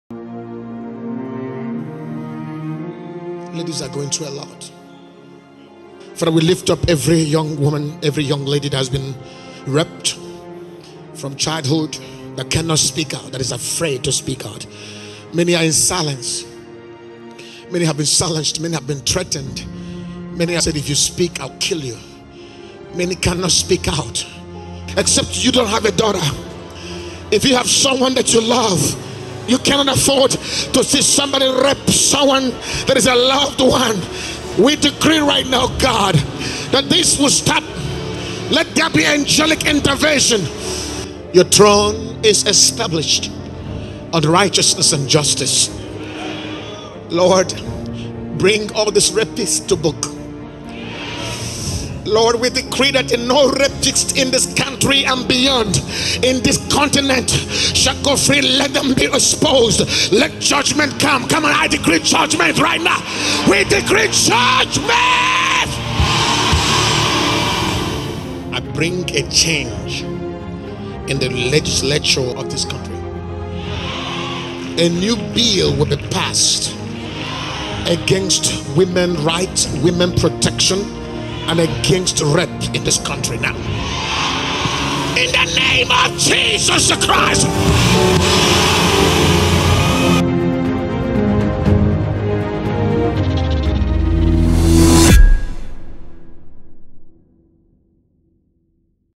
PRAYER AGAINST RAPE AND WOMEN HARASSMENT IN SOUTH AFRICA
Pastor John has been concerned and moved by the rate of crimes against women, rape and harassment happening in our country and the city of Cape Town. On 4th September 2019, the man of God was moved by compassion  and led by the Spirit of God to pray for the women and young girls. He released Angels of protection and judgement to sweep across the city and the continent of Africa.

No pediphile will go unpunished, No rapist shall escape this prayer. The judgement of God has been released. Enough of missing girls and rape cases in our country. The tears of women shall not go unnoticed. If you are a rapist, repent now or brace yourself to face the judgement of God.

If you are a victim of rape, our prayers are with you. Be encouraged, your life is not over yet. Stand strong and speak out